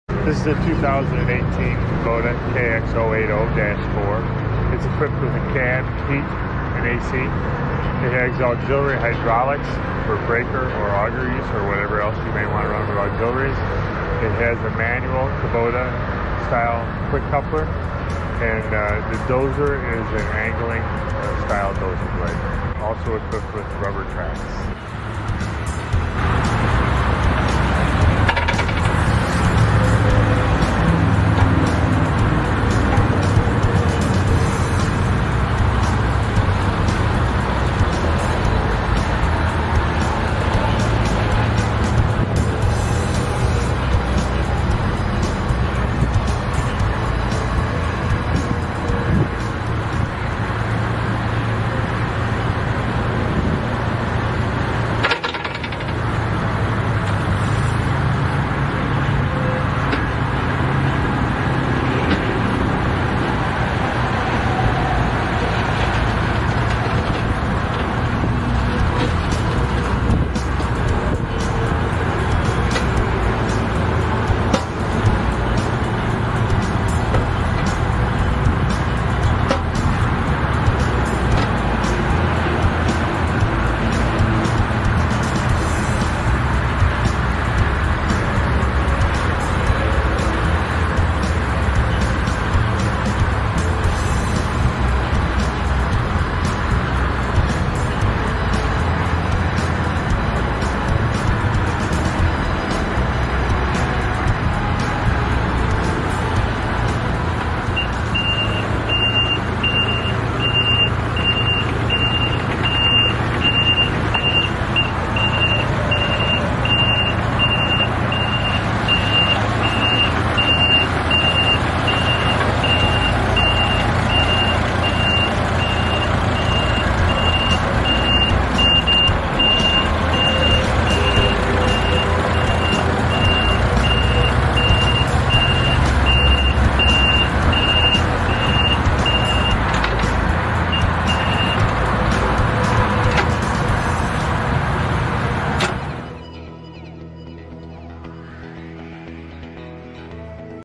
2018 Kubota KX040-4 For Sale
For Sale or Rent Used 2018 Kubota KX040-4 Excavator-Mini On Yard serial#  stock# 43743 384 hours
43500.00 USD
Martin Implement Sales Inc. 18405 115th Ave. Orland Park Illinois USA 60467
• Kubota KX040-4 mini-excavator, model KX040-4R3A, with rubber tracks, cab with heater & A/C, angling dozer blade, dipper arm, counterweight & travel alarm .
• Stock #: 43743
• Category: For Sale or Rent
• Status: On Yard
• Class: Used
• Available: Immediately
• Make: Kubota
• Model: KX040-4
• Year: 2018
• Est Hours: 384
• Fuel Type: Diesel
• Notes: Kubota KX040-4 mini-excavator, model KX040-4R3A, with rubber tracks, cab with heater & A/C, angling dozer blade, dipper arm, counterweight & travel alarm
• Transmission: Hydrostatic